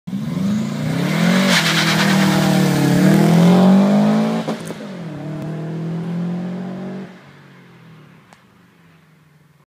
Subaru 0-100
Subaru launch with some wheel spin